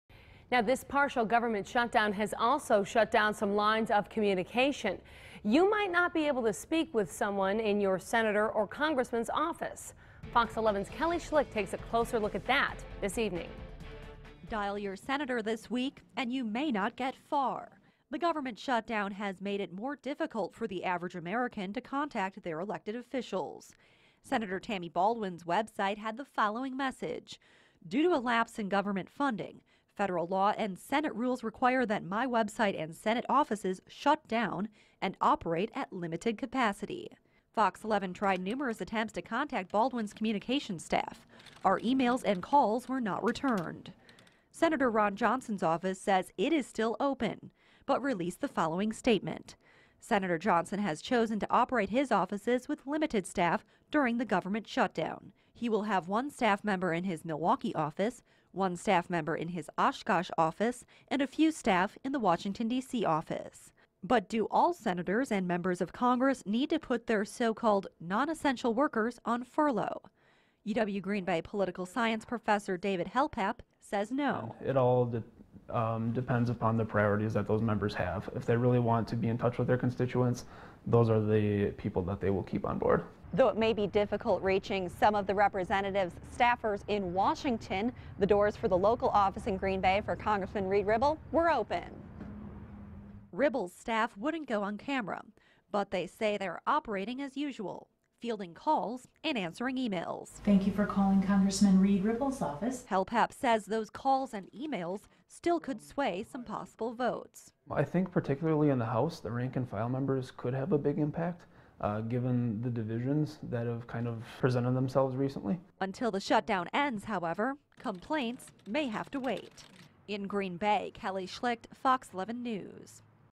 9PM Wednesday: Trouble contacting lawmakers
Dial your Senator this week, and you may not get far. The government shut down has made it more difficult for the average American to contact their elected officials.